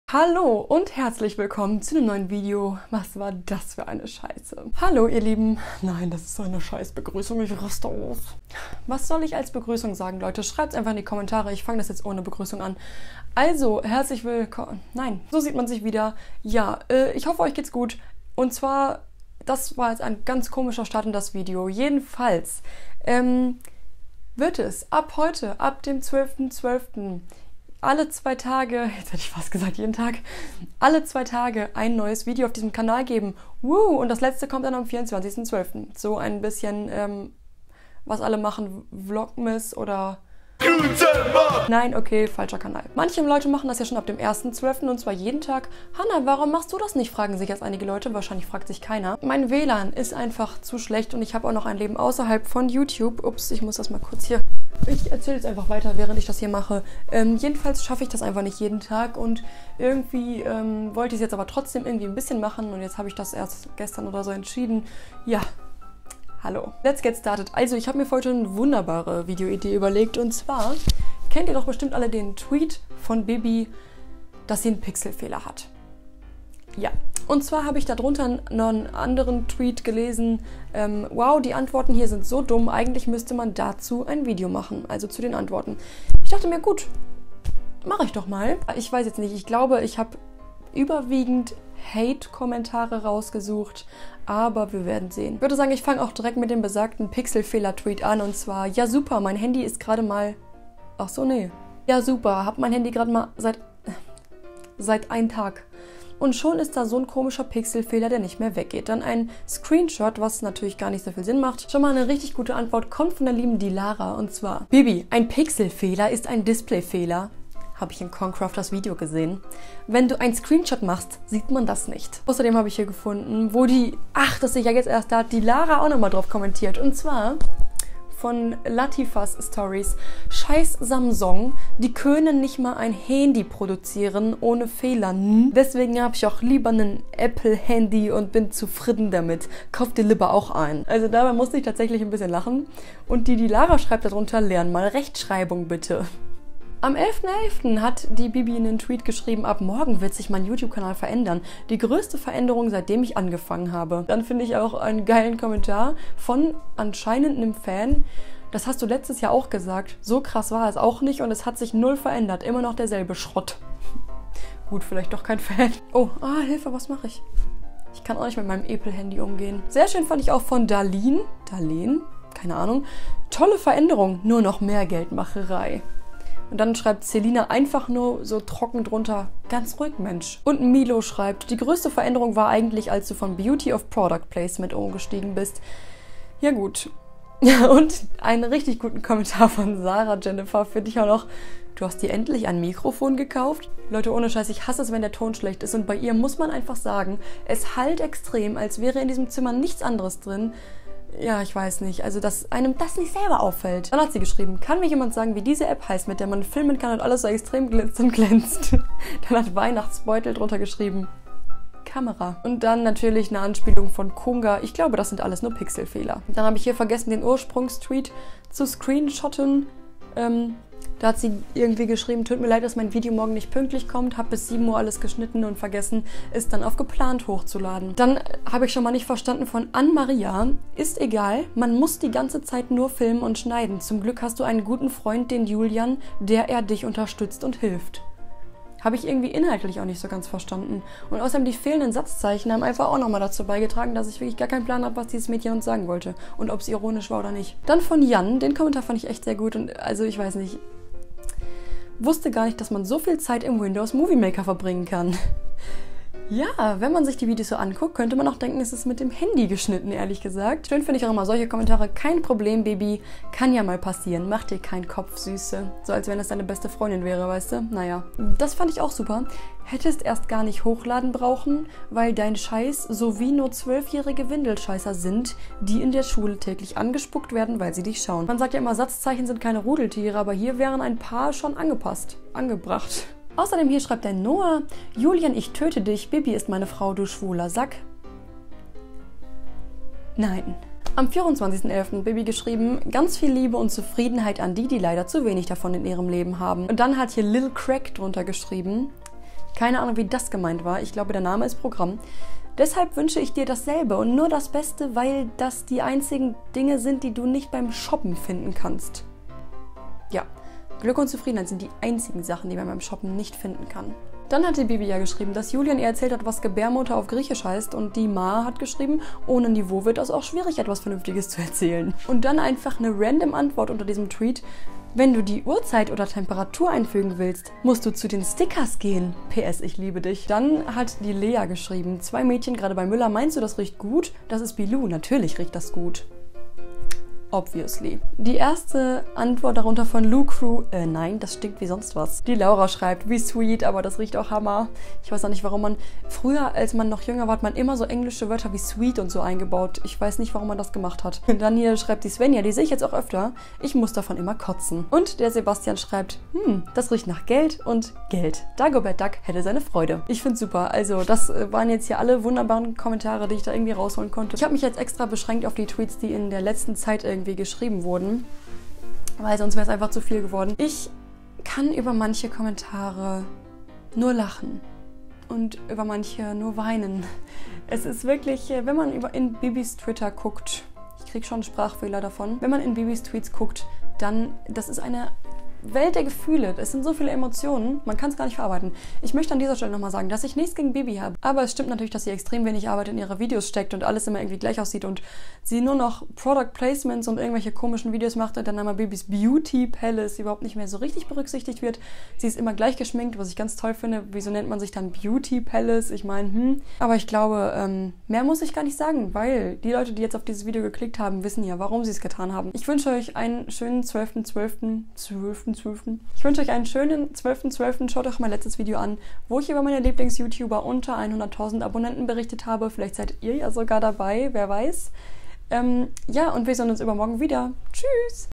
Die DÜMMSTEN/WITZIGSTEN Tweets an BibisBeautyPalace
Dieses Video soll in keinster Weise Beleidigend wirken und dient einzig und allein der Unterhaltung.

Wir sehen uns hoffentlich jeden geraden Tag bis zum 24.12! Ab dem 12.12 kommt jeden zweiten Tag ein neues Video von mir. Immer neue Themen, also wirklich für jeden ist was dabei :P

🔮Bücher Instagram: @dunkelbuntebuecherwelt

▶️Schnittprogramm: iMovie